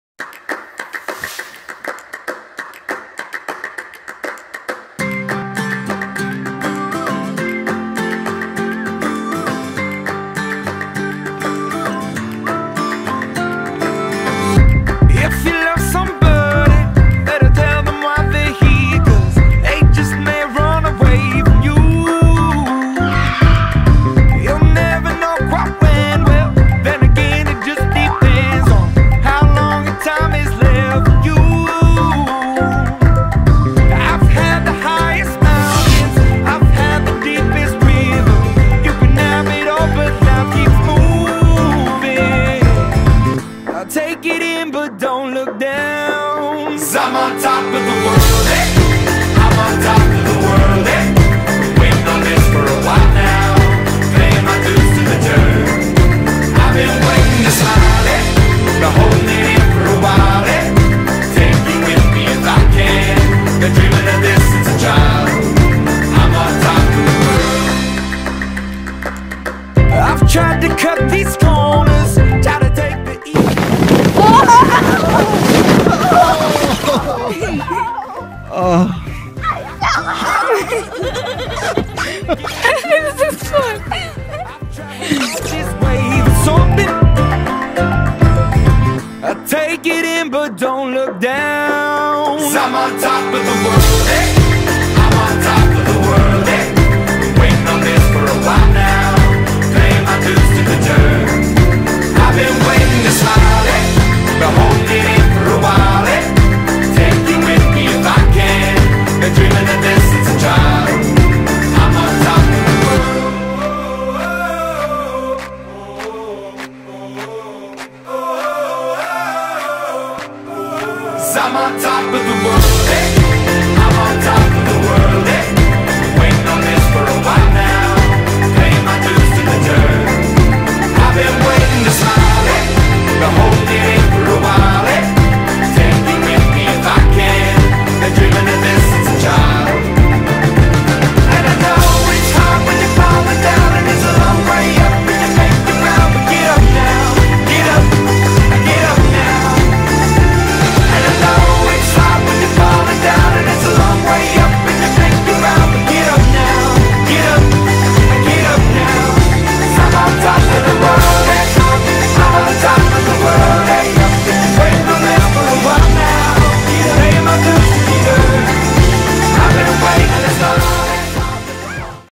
Snow Fun at All
Wargo boys peacefully enjoying the snowy winter.